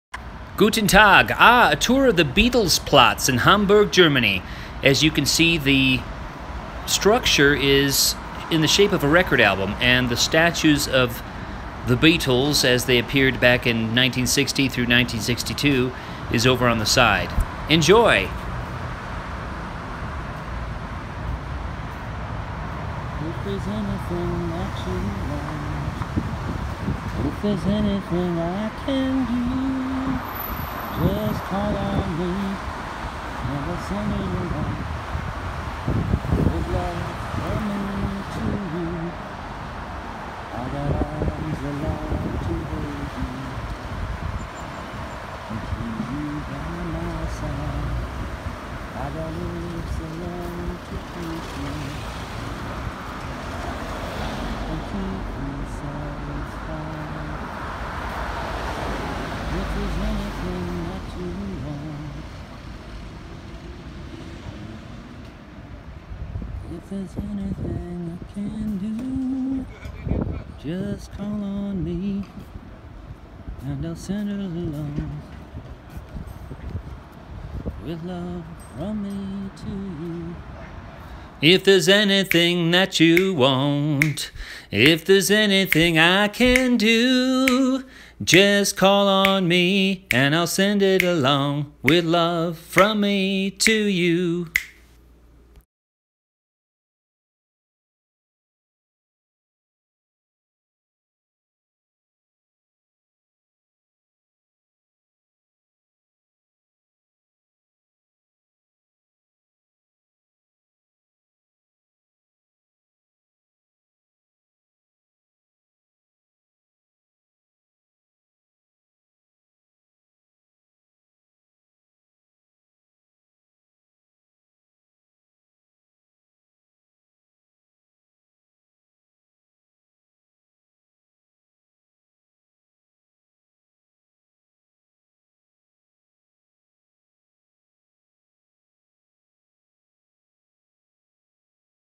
A tour of Beatles-Platz in Hamburg
A tour of Beatles-Platz in Hamburg:  History, music, geography.